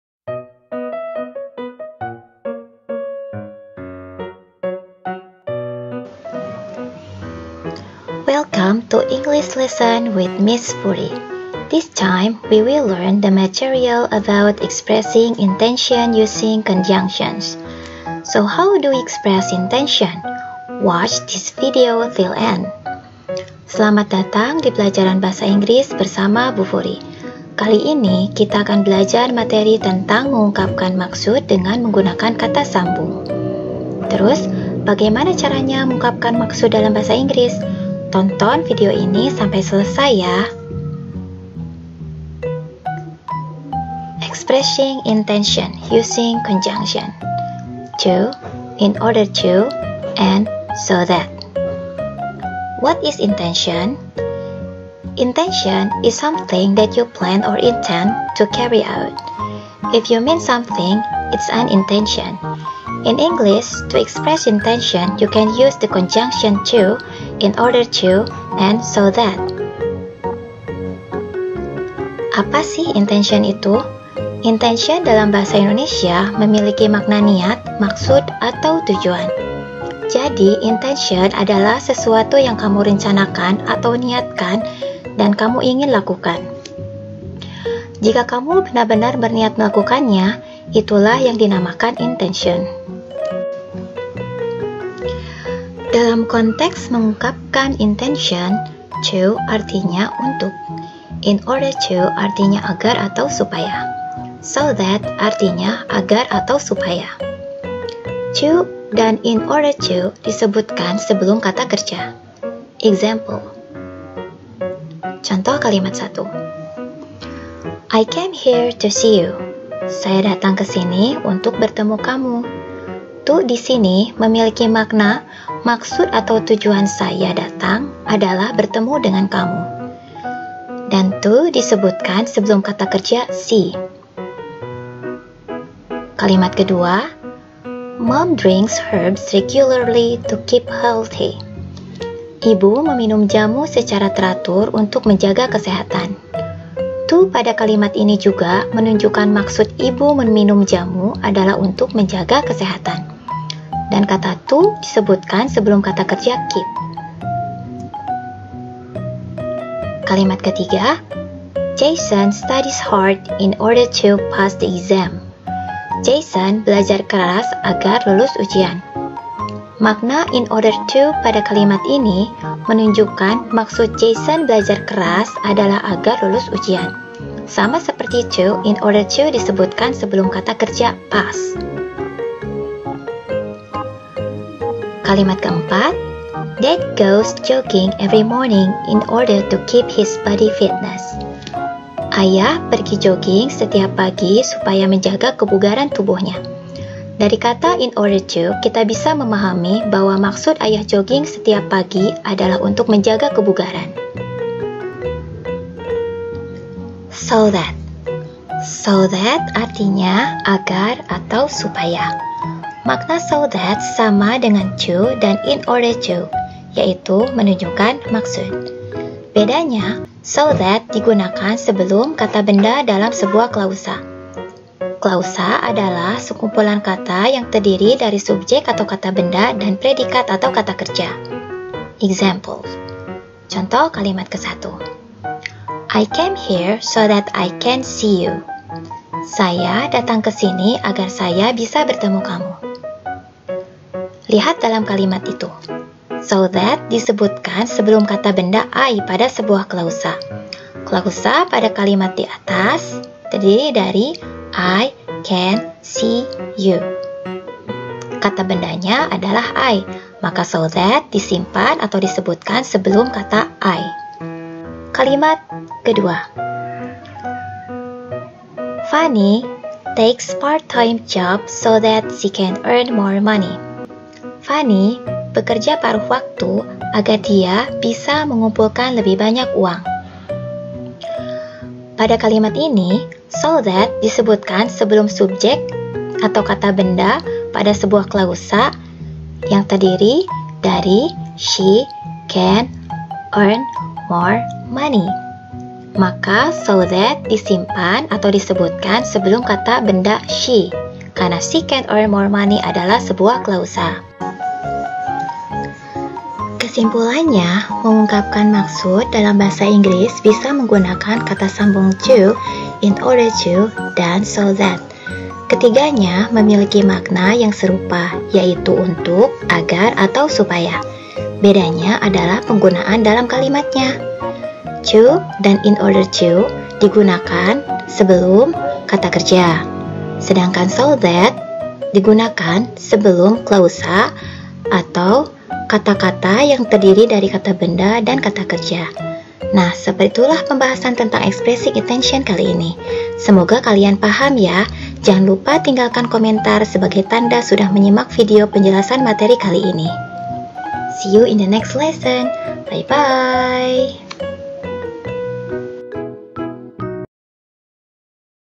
Expressing Intention using TO, IN ORDER TO, and SO THAT
Materi bahasa Inggris kelas 9.

What is expressing intention?
Ungkapan maksud/tujuan itu apa sih?

Silakan simak video penjelasannya. Mohon maaf jika kualitas video nya kurang maksimal, seperti tulisannya kurang jelas atau ada suara-suara bising yang ikut terekam.
Semoga penjelasan dalam video ini  bermanfaat bagi siapa pun yang menyimaknya.

Khusus untuk siswa siswi yang saya ajar di kelas 9 MTs Al-Hidayah Cikancung, silakan jawab 5 soal quiz berikut ini sebagai tanda kalian sudah menyimak video nya.

Complete the following sentences with to, in order to or so that.

1. I work overtime earn more money.
2. Sari set an alarm on my phone she will not oversleep.
3. I sleep earlier tonight get up before dawn tomorrow.
4. My mom will call me from her office remind me doing my homework
5. I study English I can get a better job abroad after graduating

Ketik
Nama:
Kelas:
Sekolah:

Jawaban:

di kolom komentar.

Terimakasih